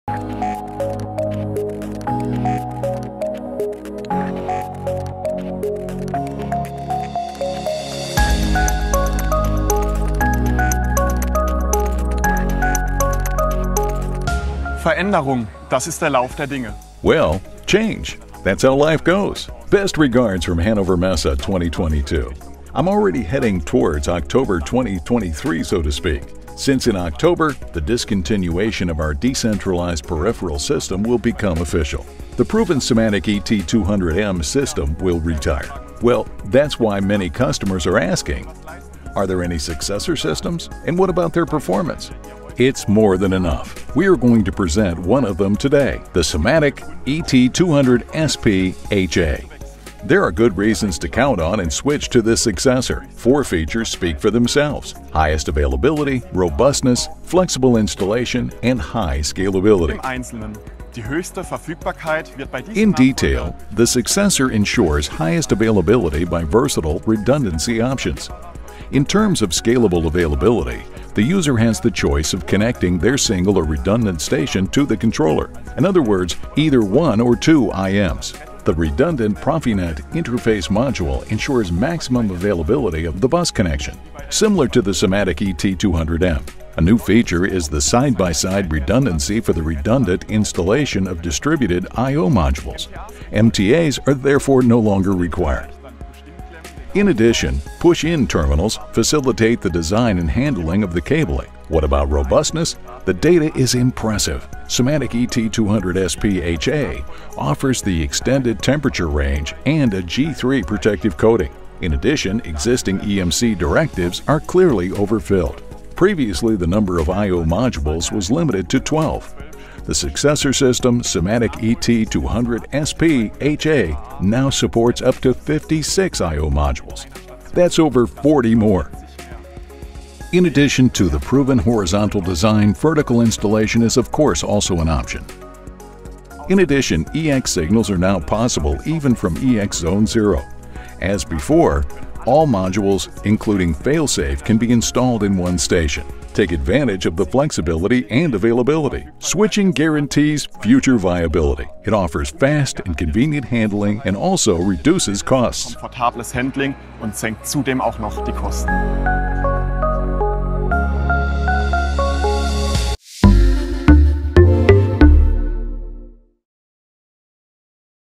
Conhecendo a remota de campo SIMATIC ET 200SP HA
A SIMATIC ET 200SP HA veio para substituir a ET200M.
Alta disponibilidade e escalabilidade, robustez e instalação flexível são apenas algumas das razões pelas quais a SIMATIC ET200SP HA é a escolha certa para as indústrias de processo. Assista a este vídeo para uma demonstração de novos e melhores recursos.

👉 Se interessou ou tem alguma dúvida? Entre em contato conosco ou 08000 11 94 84